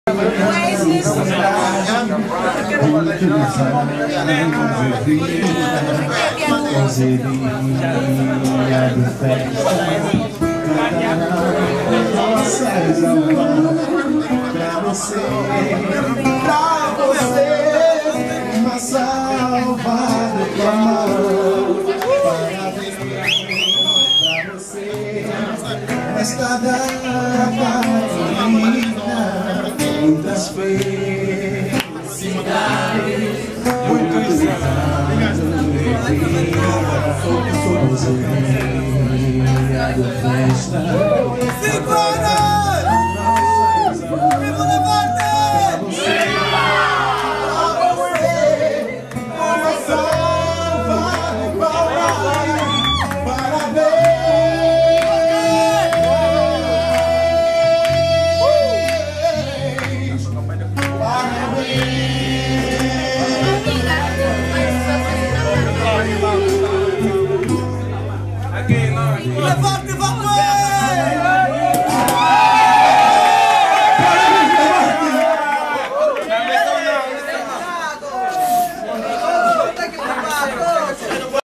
5 Velas Lev'Arte.MOV
Dia 20 de Julho de 2011 fizemos CINCO ANOS desde que em 2006 houve a coragem para começar a Fazer Acontecer Arte com a Magia das Palavras. Este ano de 2011 celebramos no King's Club na "Quinta da POESIA EU ViVo" com muita alma e arte nas PALAVRAS.